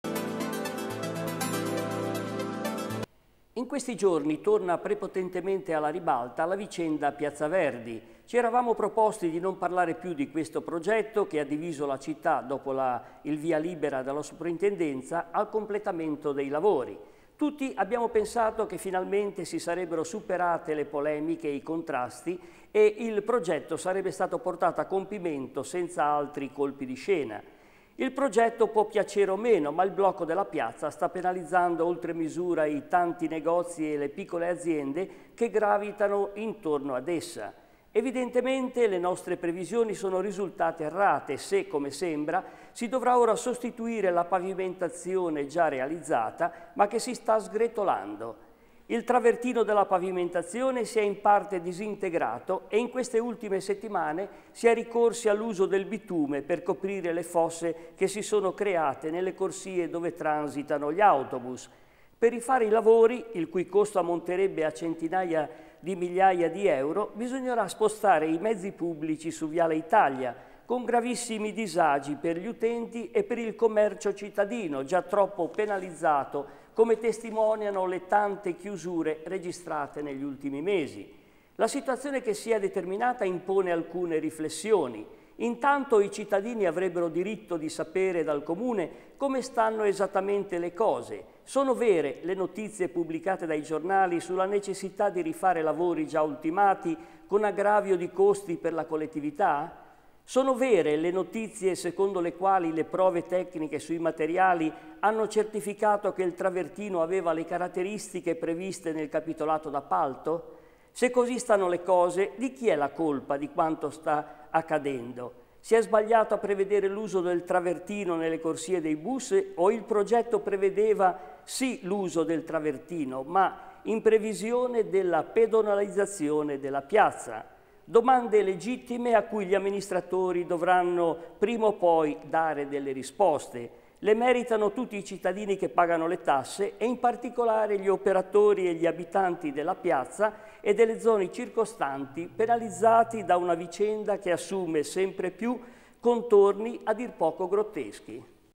Piazza Verdi bus in viale italia per rifare le corsie 10-06-2016
Per rifare le corsie degli autobus da poco completate in piazza Verdi si dovranno spostare gli autobus in viale Italia. Una decisione che dovrà essere assunta nei prossimi giorni e che creerà sicuramente polemiche tra i cittadini e soprattutto tra i commercianti della piazza